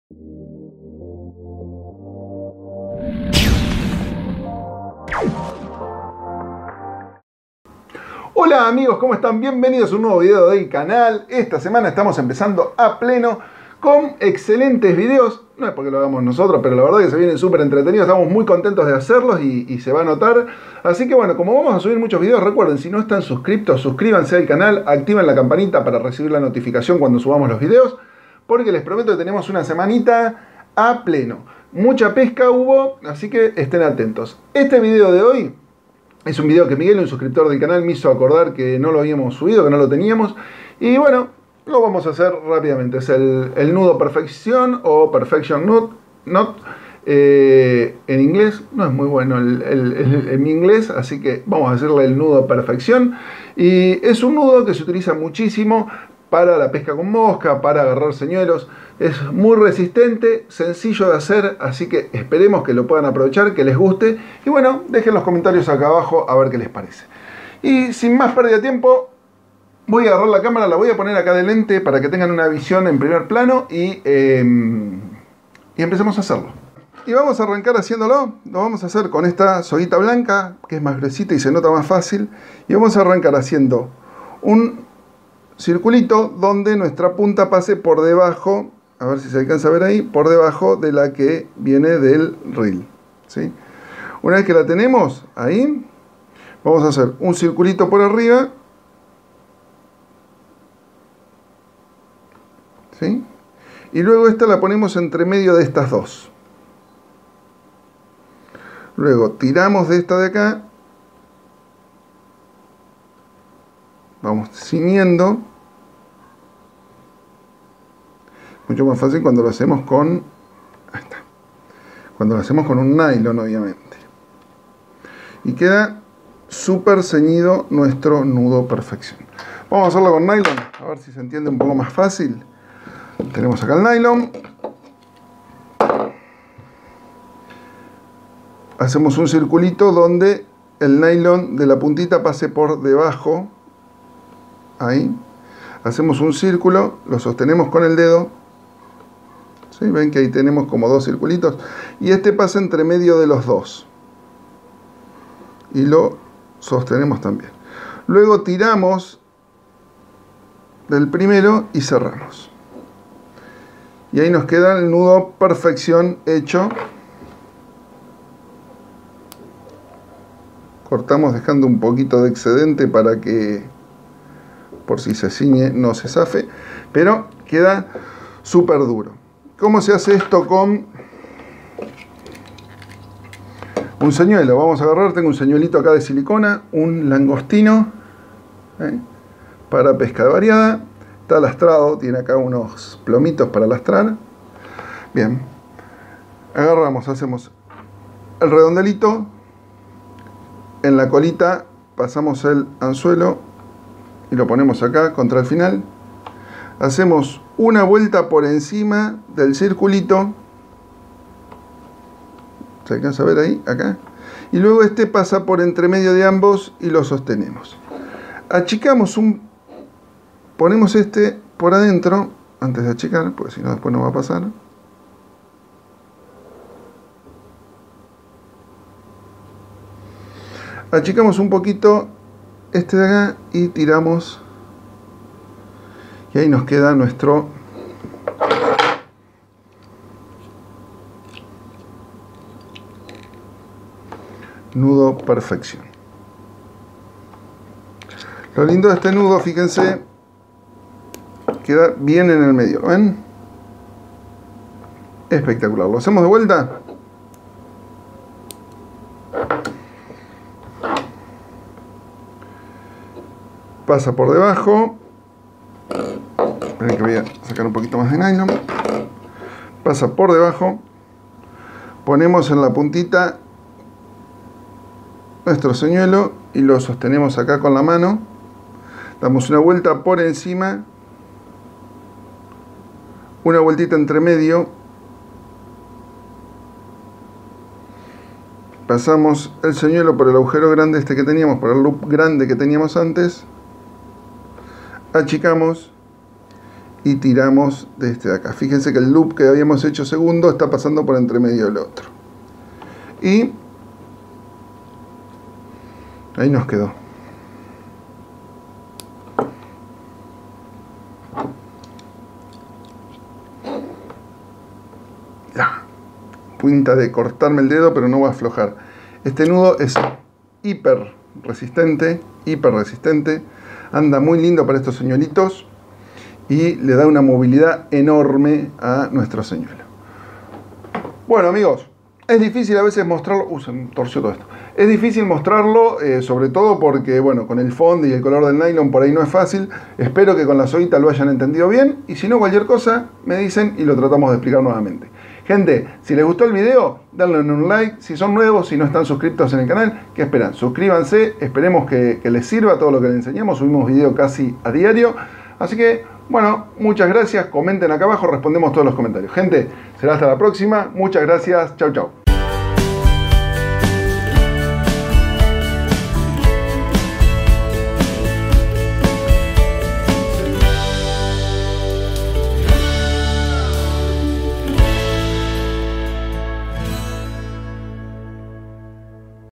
Nudo perfección - Perfection loop knot
Perfection loop knot - Nudo Perfección
Este nudo tiene la particularidad de poder atarse en lineas que van de los 2 a las 600 libras con un ajuste perfecto. Utilizado en pesca con mosca y señuelos demuestra una utilidad y sencillez asombrosa!